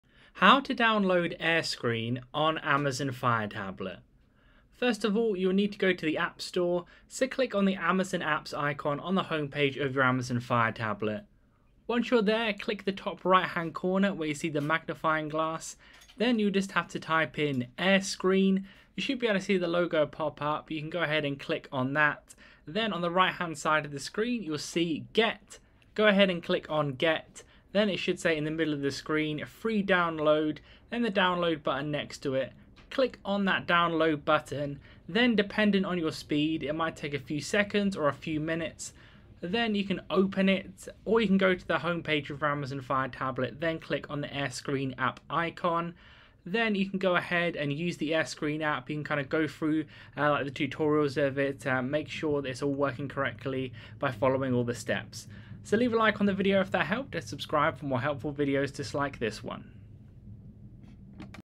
How to Download AirScreen on Amazon Fire Tablet
How to Download AirScreen on Amazon Fire Tablet. Step-by-step instructions on how to install air screen on your amazon tablet. This tutorial shows you can get and use airscreen app on any version of the amazon fire tablet in 2022.